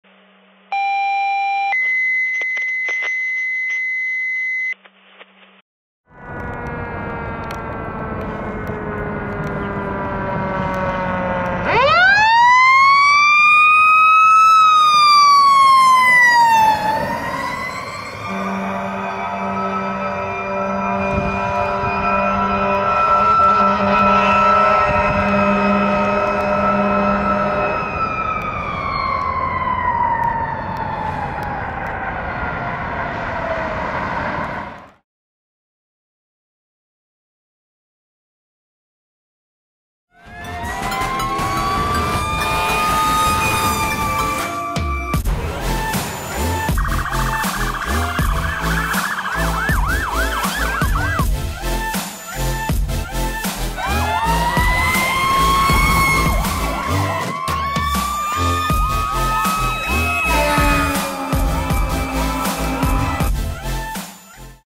Brandywine Hundred Fire Company Tower 11 Responding 12/21/21
Brandywine Hundred Fire Company Tower 11 Responding in Egemoor to Box in New Castle County Delaware